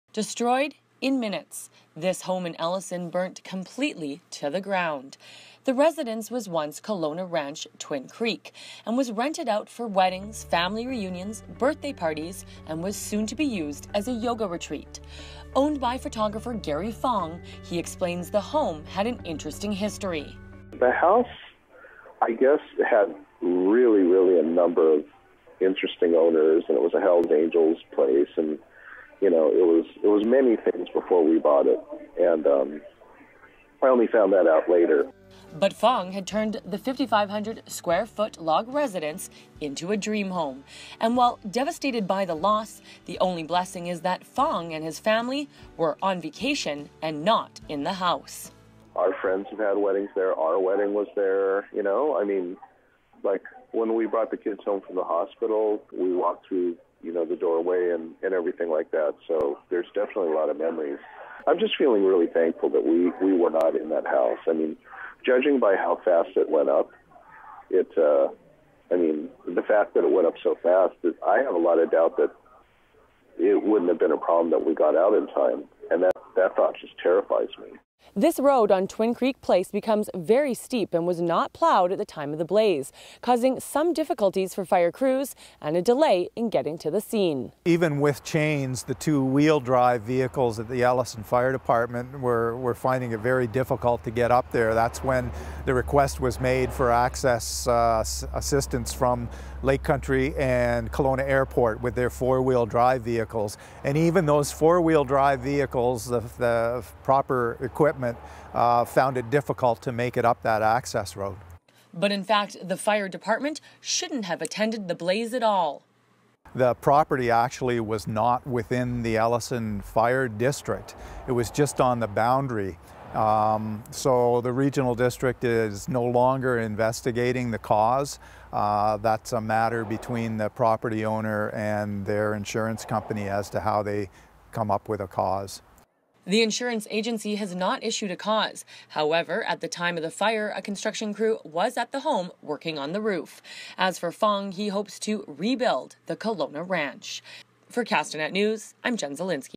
Gary Fong's Ranch House Fire
Gary, Melissa, Wesley and Willow Fong are safe, in California on a holiday.